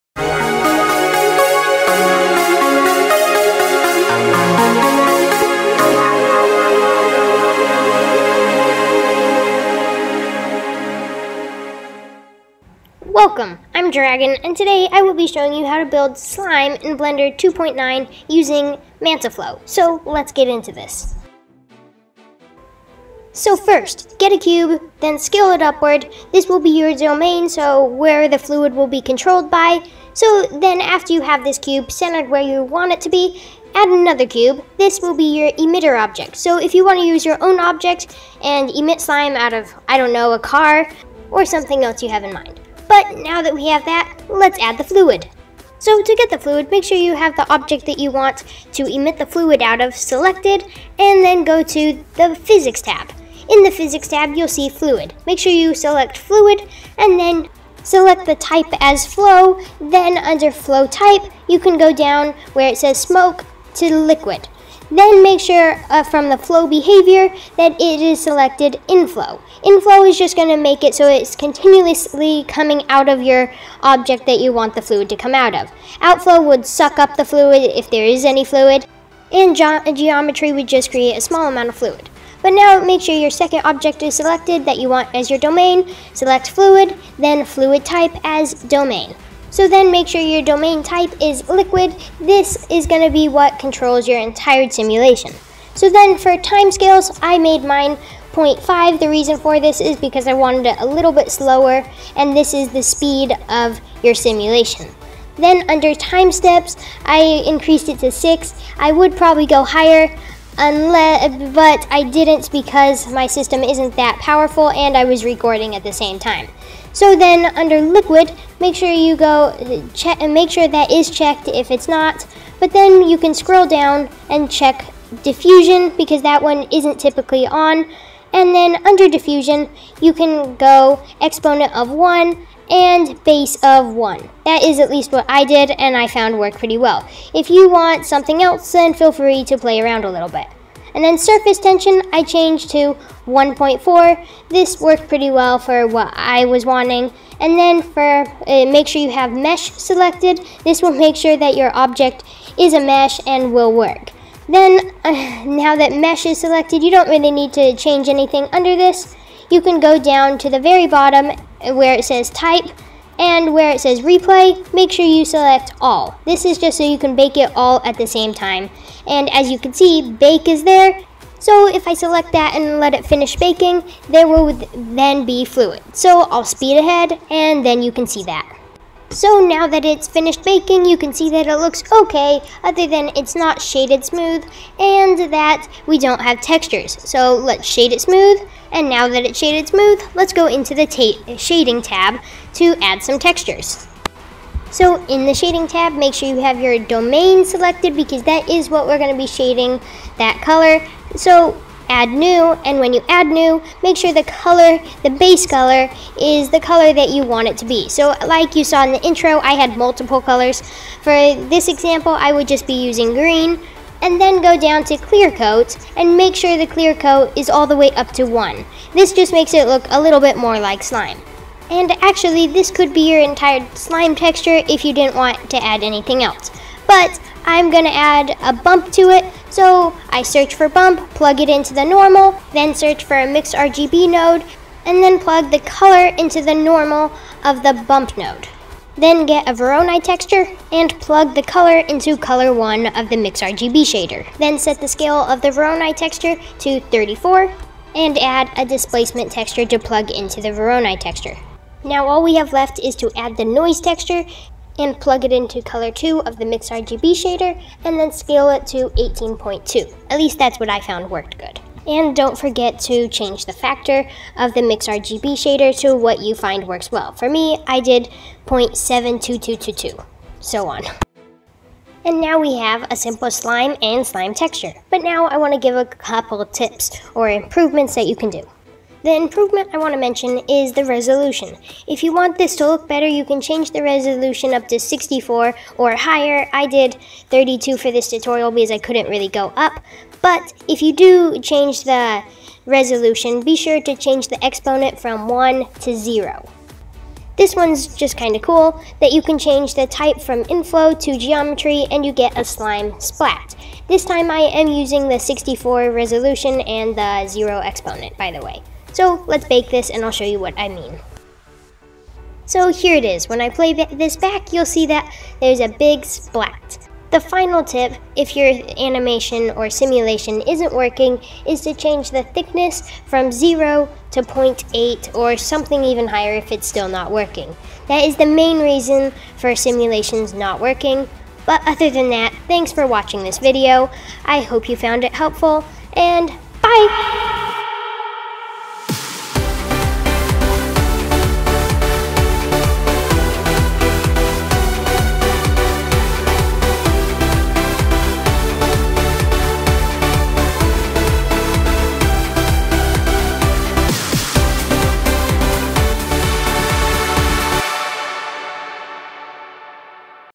Slime Tutorial for Blender 2.9 (Liquid Simulation)
Today I will be making a slime tutorial for blender 2.9. I hope this allows you to make some cool slimy liquid simulations.

Enjoy :D